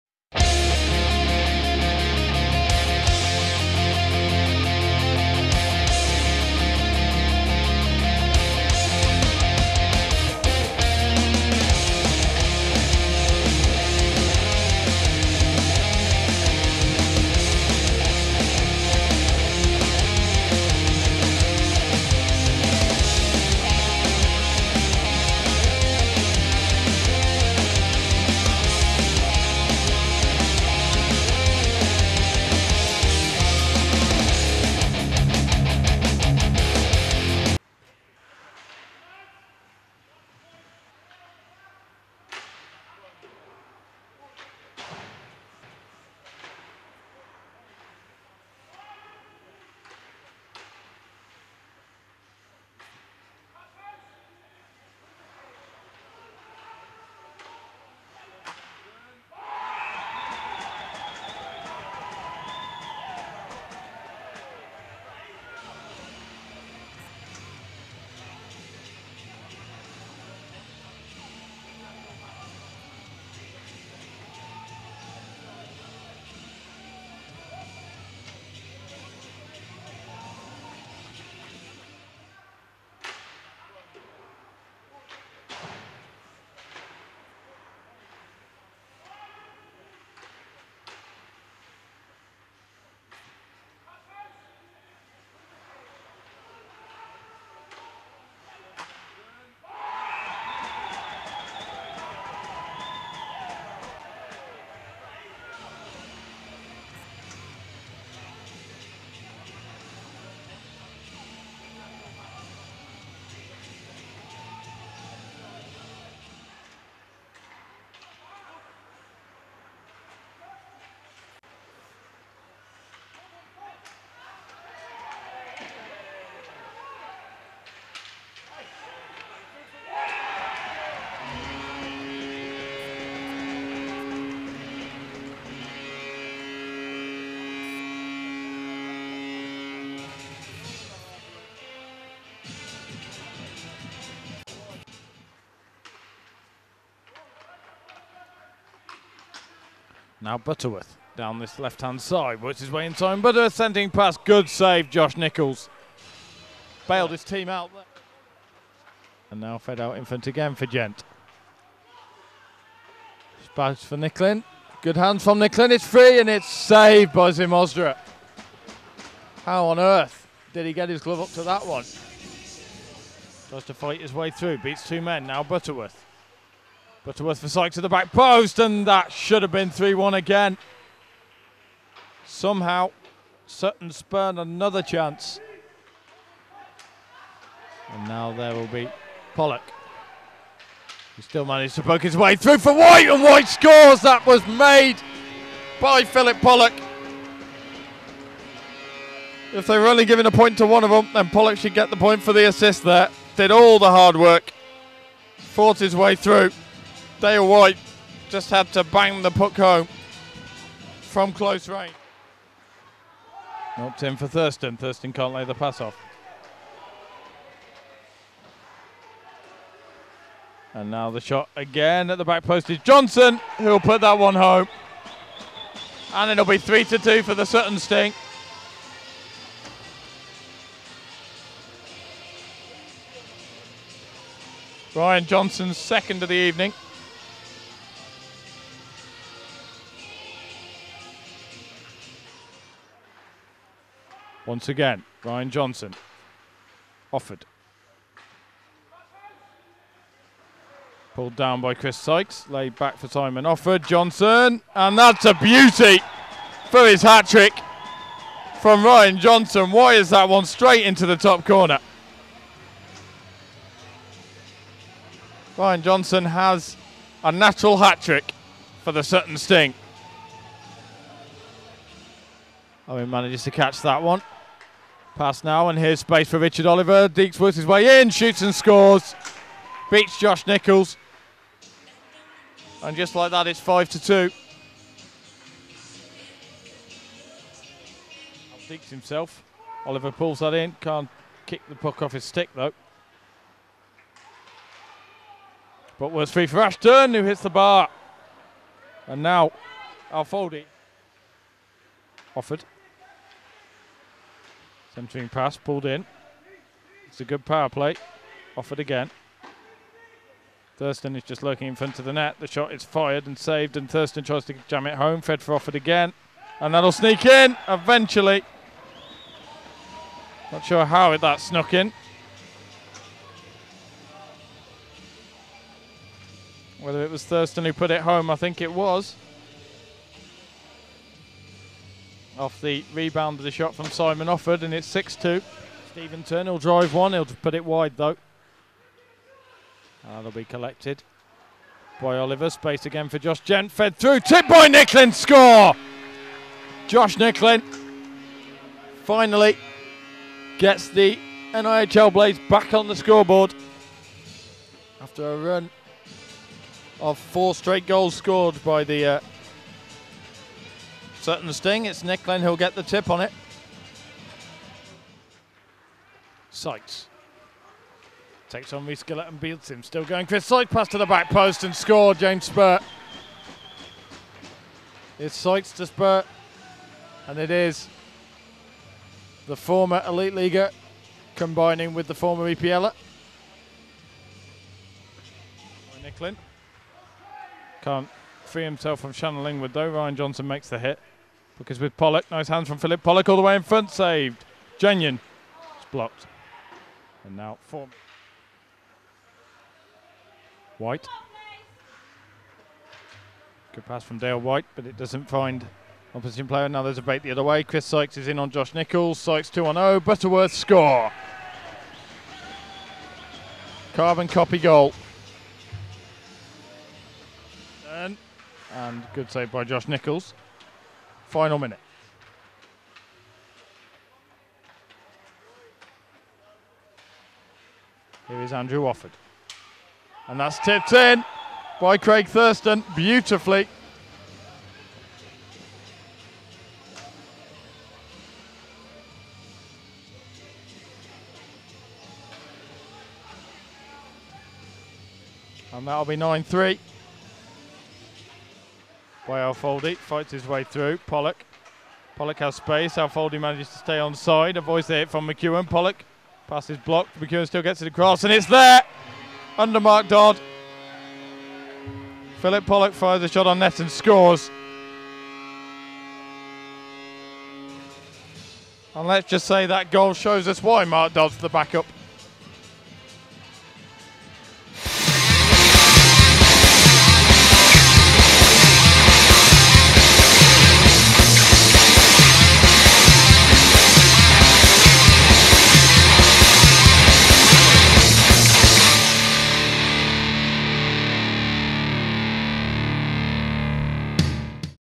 NIHL Coventry Blaze V Sutton sting 26/01/13
Apologise for the late upload, as always our producers life has been hectic of late.

We seem to have a jinx against Sutton as our camera ate the audio for the first period, and made the tape unplayable on anything but an old Panasonic camera.

Thanks to Pete Ballinger for filling in on camera while Chris was off celebrating his birthday